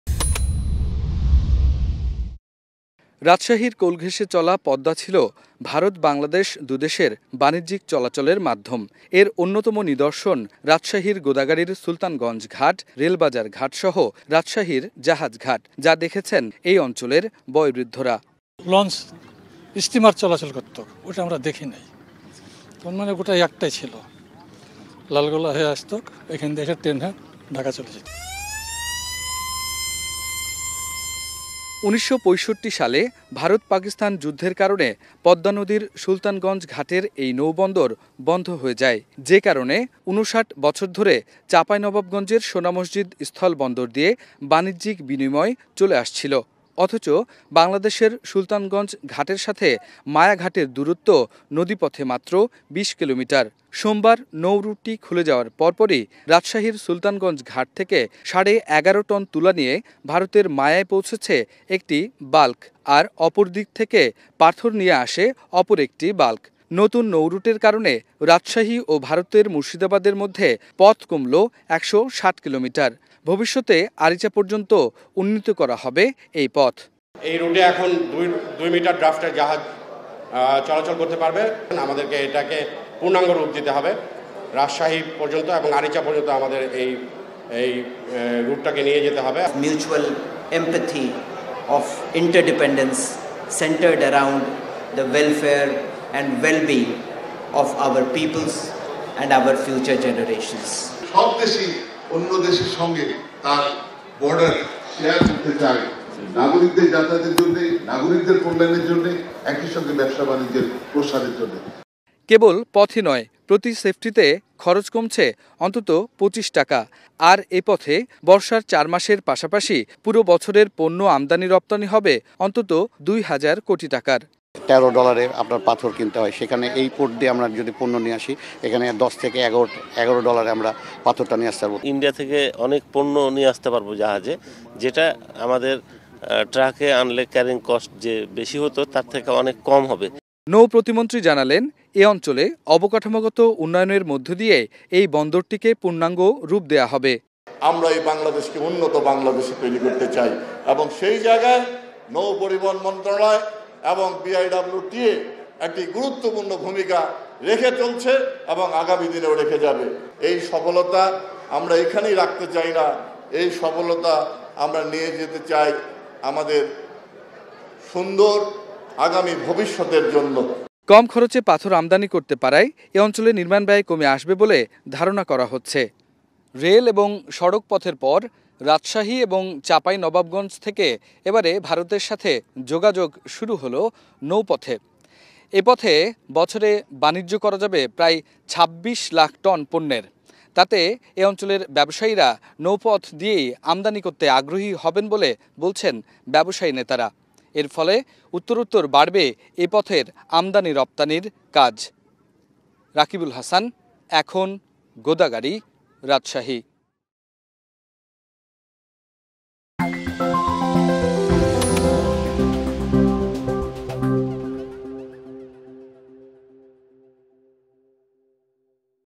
কমবে আমদানি রপ্তানি ও পণ্য পরিবহন ব্যয় | Rajshahi to Murshidabad | Export Import | Ekhon TV
Our Office Address:
EKHON TV
City Park Lane
19, Hatkhola Road, Wari
Dhaka-1203
Bangladesh